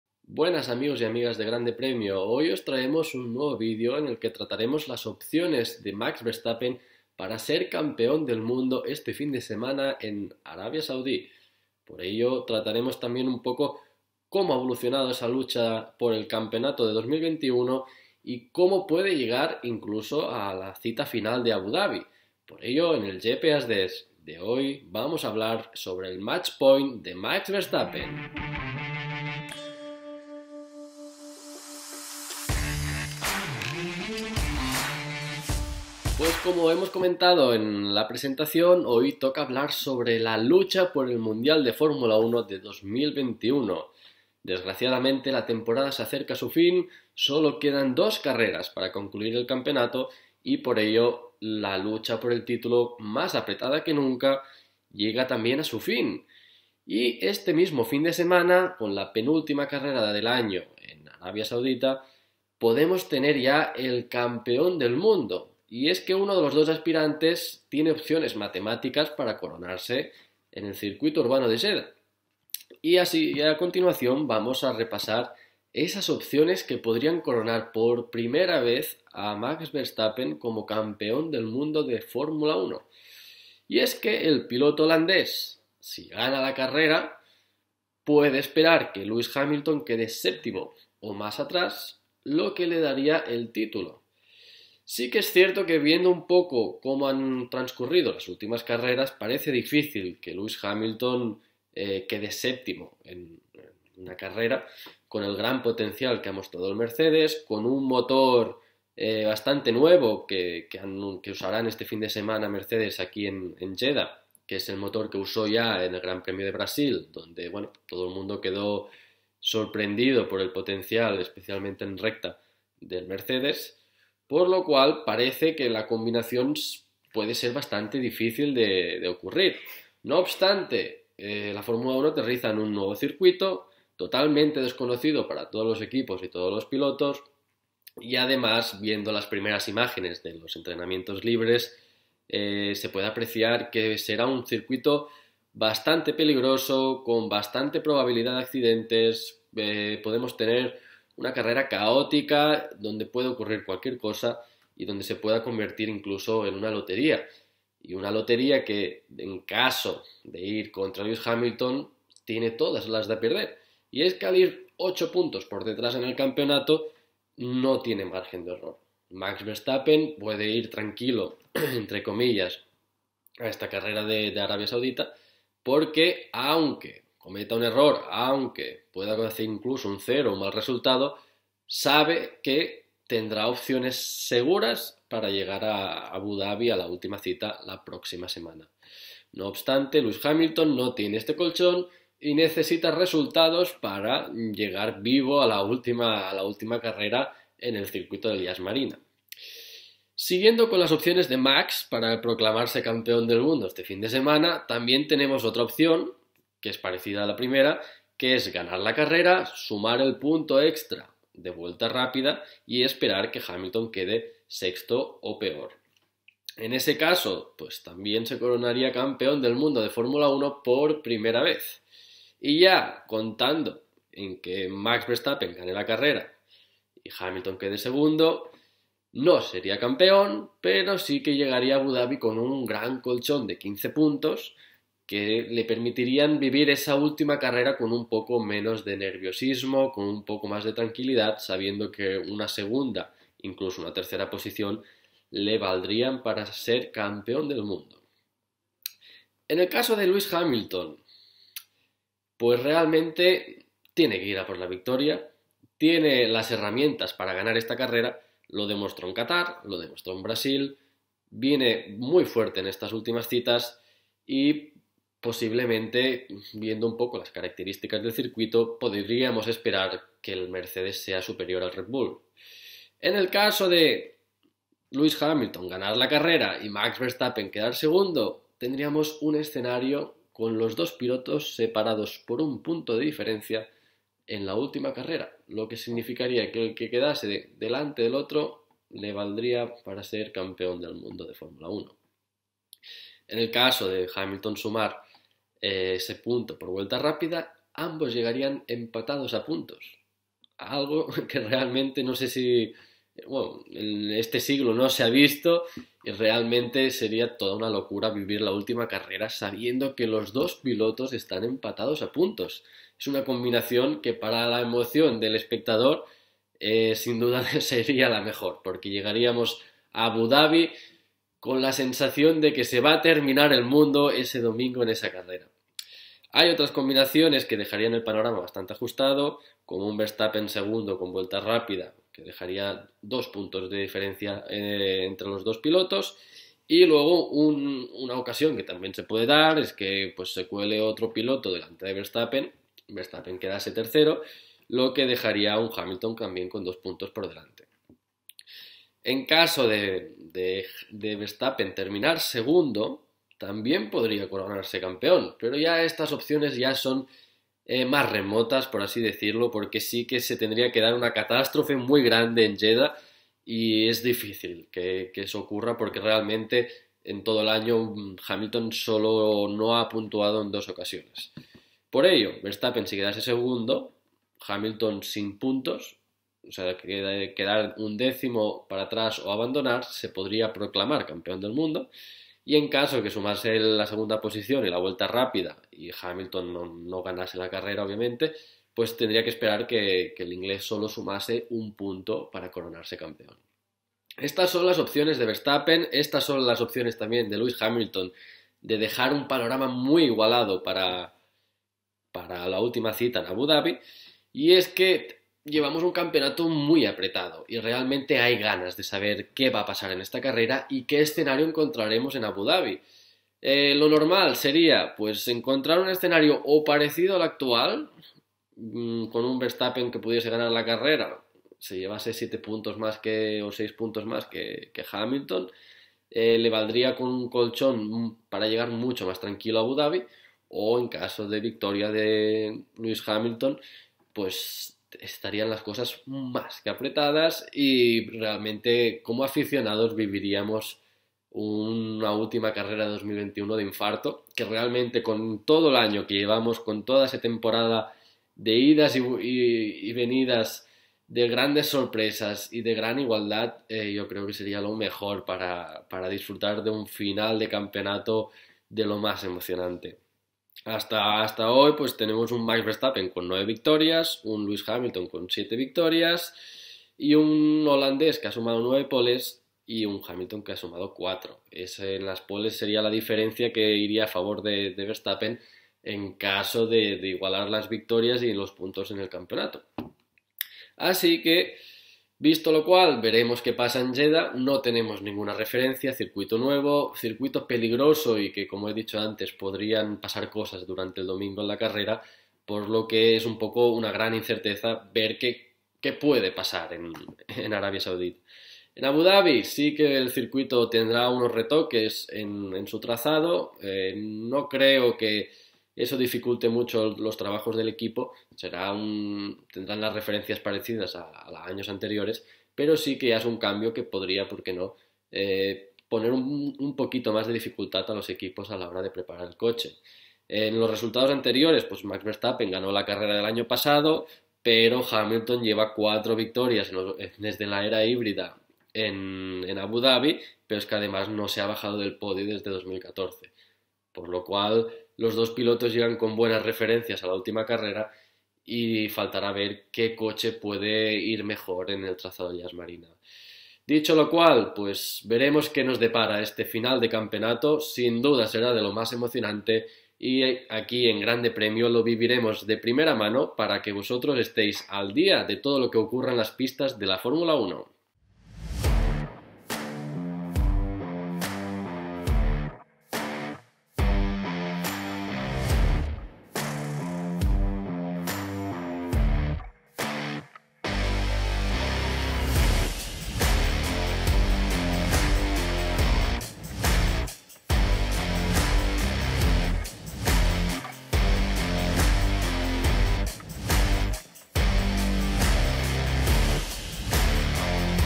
MAX VERSTAPPEN Y SU PRIMER MATCH-POINT EN JEDDAH PARA CONVERTIRSE EN CAMPEÓN DE F1
Max Verstappen se enfrenta este fin de semana en Arabia Saudita con la primera oportunidad de su vida para convertirse en campeón del mundo de F1. El holandés llega con ocho puntos de ventaja sobre Lewis Hamilton a falta de dos carreras para la conclusión del campeonato.

Eric Calduch analiza las diferentes combinaciones matemáticas que se podrían dar en Jeddah para que Verstappen se proclame campeón del mundo. Además, también repasará las posibilidades de Hamilton para alargar la igualada decisión para Abu Dabi.